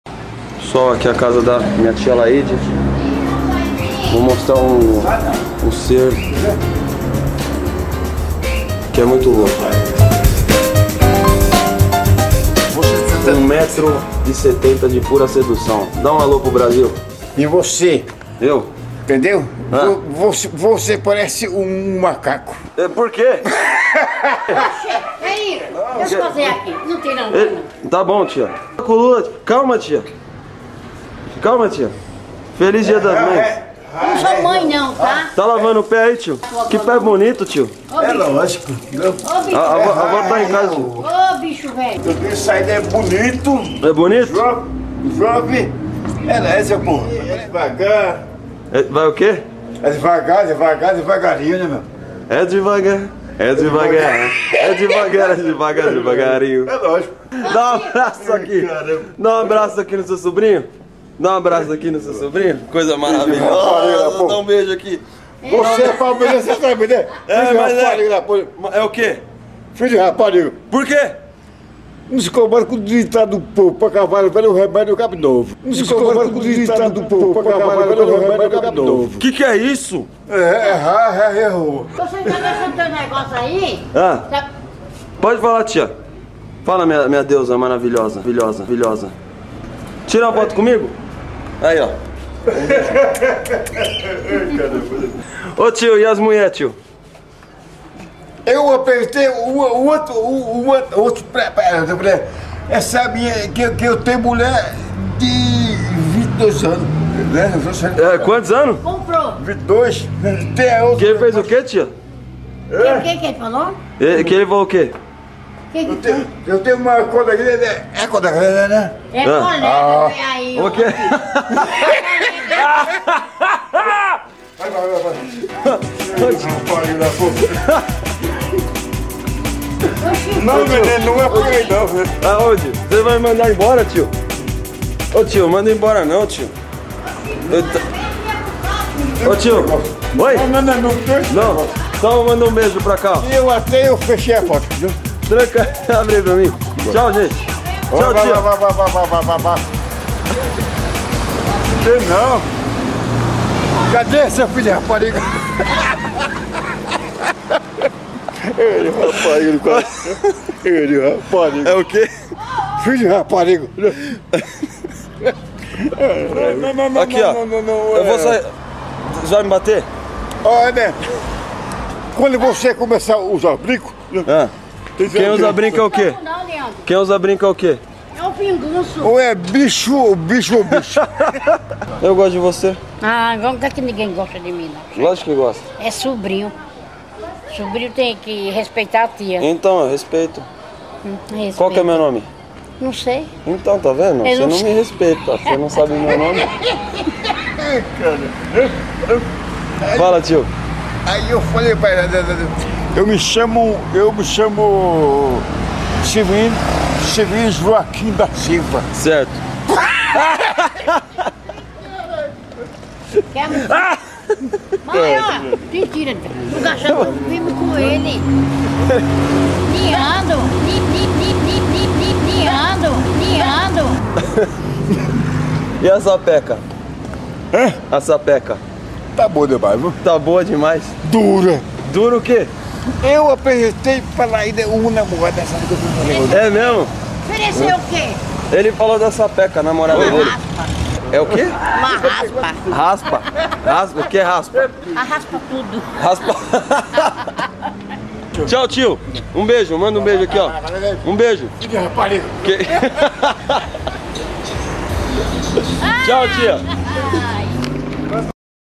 Leandro MATOZO apresenta: DonaLaide e Tio Chico
Apresento a vocês meu tio que é viciado em água mineral natural e minha tia que é a coisa mais fofinha do mundo.